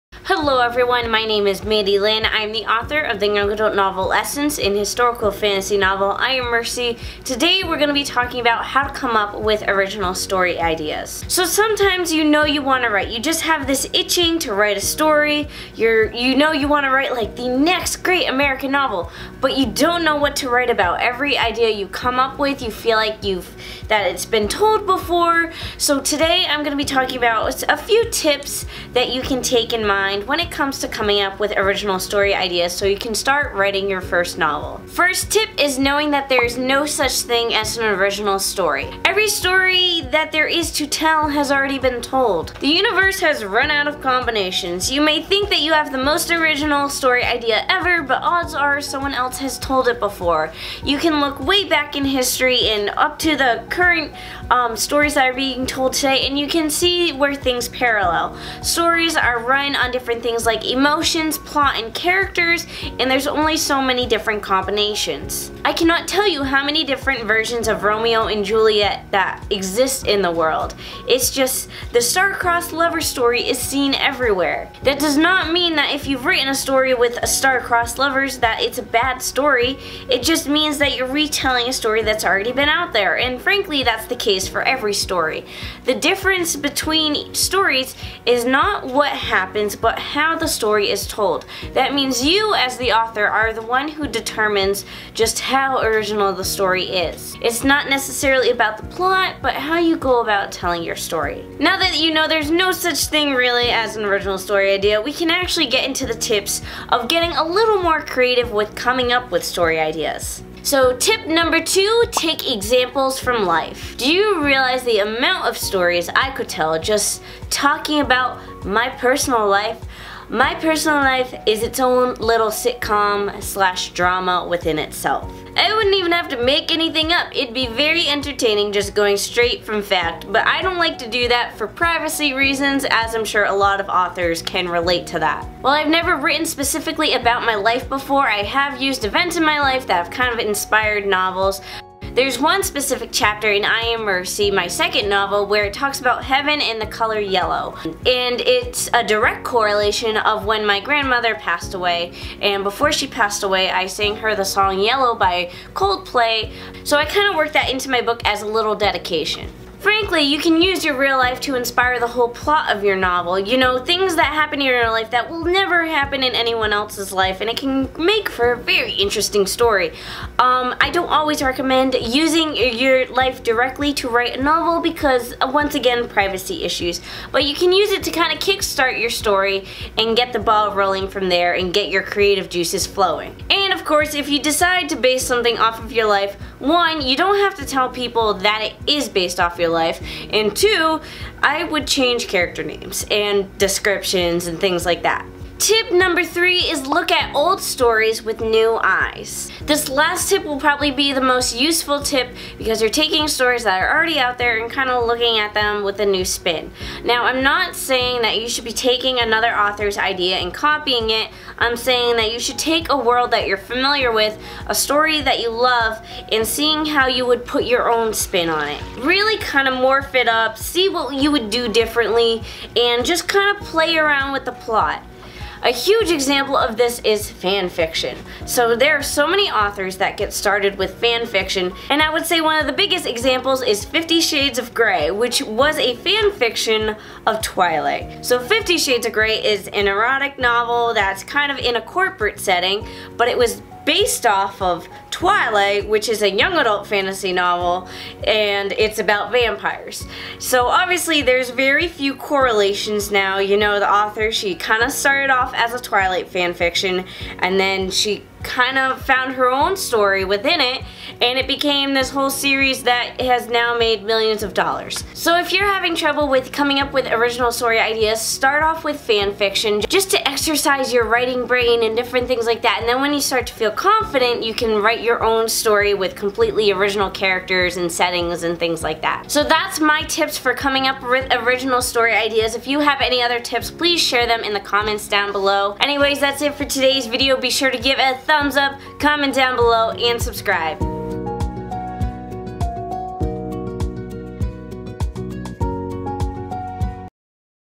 How to Come Up With Original Story Ideas - Novel Writing
Sometimes you know that you want to write, but you have no idea what you want to write about. You’re ready to start writing the next great American novel, but every idea you come up with seems like it’s been told before. In today’s blog, I’m going to cover a few different tips that you can use to help come up with an original story idea that you can use to start writing a novel.

→Marketing for Authors Series
→I am Mercy